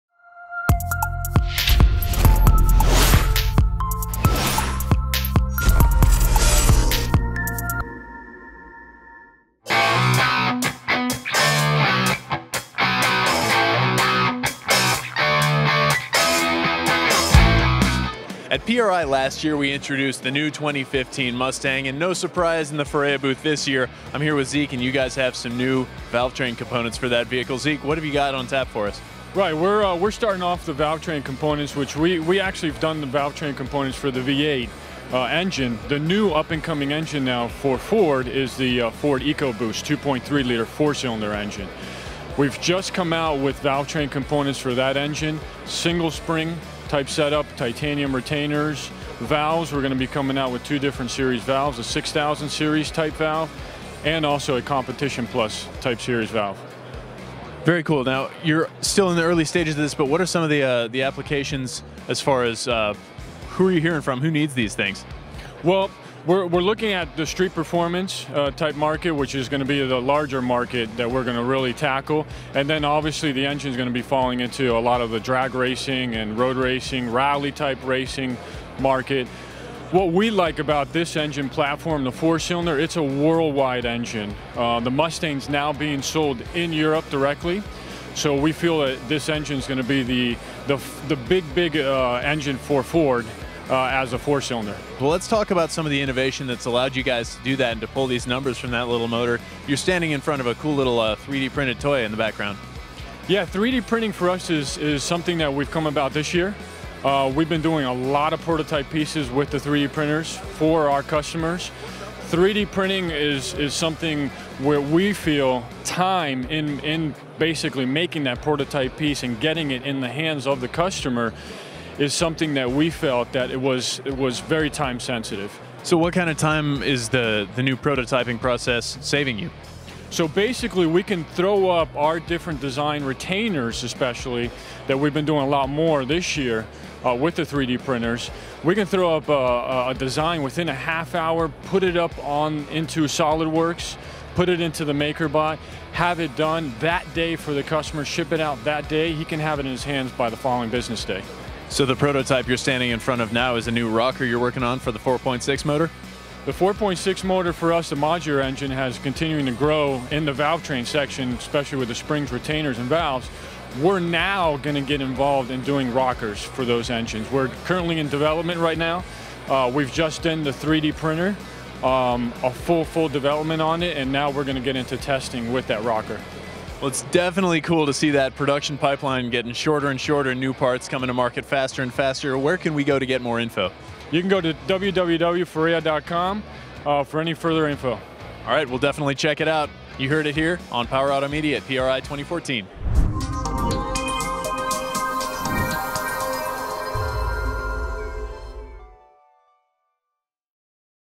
PRI 2014: Ferrea's new Valvetrain Components Offerings for the 2.3 Liter Ecoboost Engines
Two different types of valves, titanium retainers, and single spring set ups for the Ford 2.3L 4 Cylinder Ecoboost engine are what's on tap for Ferrea for 2015.